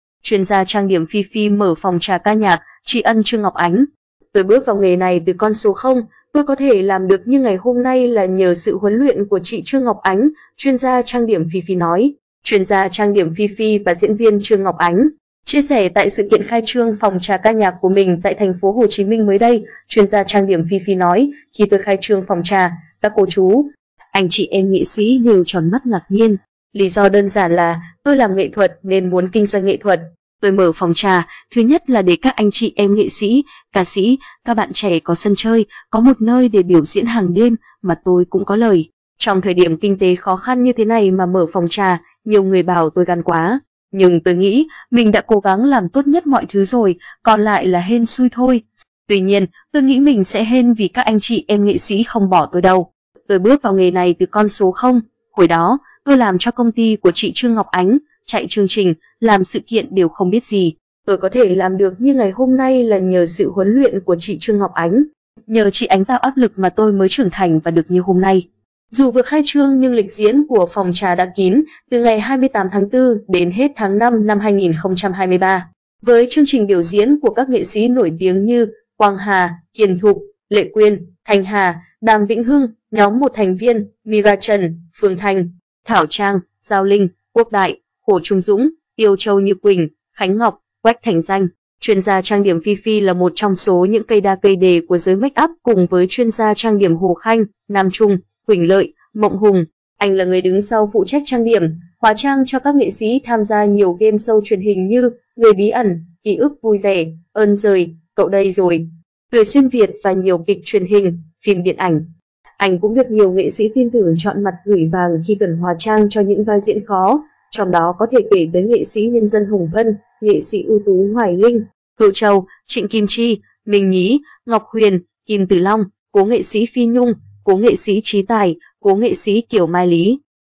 Chuyên gia trang điểm Phi Phi mở phòng trà ca nhạc, tri ân Trương Ngọc Ánh
'Tôi bước vào nghề này từ con số 0. Tôi có thể làm được như ngày hôm nay là nhờ sự huấn luyện của chị Trương Ngọc Ánh', chuyên gia trang điểm Phi Phi nói.Chuyên gia trang điểm Phi Phi và diễn viên Trương Ngọc Ánh
Chuyên gia trang điểm Phi Phi là 1 trong số những "cây đa cây đề" của giới make-up cùng với chuyên gia trang điểm Hồ Khanh, Nam Trung, Huỳnh Lợi, Mộng Hùng...
Tôi bước vào nghề này từ con số 0. Hồi đó, tôi làm cho công ty của chị Trương Ngọc Ánh, chạy chương trình, làm sự kiện đều không biết gì. Tôi có thể làm được như ngày hôm nay là nhờ sự huấn luyện của chị Trương Ngọc Ánh. Nhờ chị Ánh tạo áp lực mà tôi mới trưởng thành và được như hôm nay".
Anh là người đứng sau phụ trách trang điểm, hóa trang cho các nghệ sĩ tham gia nhiều game show truyền hình như: Người bí ẩn; Ký ức vui vẻ; Ơn giời, cậu đây rồi; Cười Xuyên Việt... và nhiều kịch truyền hình, phim điện ảnh.
Chuyên gia trang điểm Phi Phi cùng ca sĩ Hiền Thục
Trong thời điểm kinh tế khó khăn như thế này mà mở phòng trà, nhiều người bảo tôi gan quá. Nhưng tôi nghĩ, mình đã cố gắng làm tốt nhất mọi thứ rồi, còn lại là hên xui thôi. Tuy nhiên, tôi nghĩ mình sẽ hên vì các anh chị em nghệ sĩ không bỏ tôi đâu.
Dù vừa khai trương nhưng lịch diễn của phòng trà đã kín từ ngày 28/4 đến hết tháng 5/2023 với chương trình biểu diễn của các nghệ sĩ nổi tiếng như: Quang Hà, Hiền Thục, Lệ Quyên, Thanh Hà, Đàm Vĩnh Hưng, nhóm MTV, Myra Trần, Phương Thanh, Thảo Trang, Giao Linh, Quốc Đại, Hồ Trung Dũng, Tiêu Châu Như Quỳnh, Khánh Ngọc, Quách Thành Danh…
Chia sẻ tại sự kiện khai trương phòng trà ca nhạc của mình tại TPHCM mới đây, chuyên gia trang điểm Phi Phi nói: "Khi tôi khai trương phòng trà, các cô chú, anh chị em nghệ sĩ đều tròn mắt ngạc nhiên. Lý do đơn giản là, tôi làm nghệ thuật nên muốn kinh doanh nghệ thuật. Tôi mở phòng trà, thứ nhất là để các anh chị em nghệ sĩ, ca sĩ, các bạn trẻ có sân chơi, có một nơi để biểu diễn hàng đêm, mà tôi cũng có lời.
Anh cũng được nhiều nghệ sĩ tin tưởng "chọn mặt gửi vàng" khi cần hóa trang cho những vai diễn khó, trong đó có thể kể tới NSND Hồng Vân, NSƯT Hoài Linh, Hữu Châu, Trịnh Kim Chi, Minh Nhí, Ngọc Huyền, Kim Tử Long, cố nghệ sĩ Phi Nhung, cố nghệ sĩ Chí Tài, cố nghệ sĩ Kiều Mai Lý...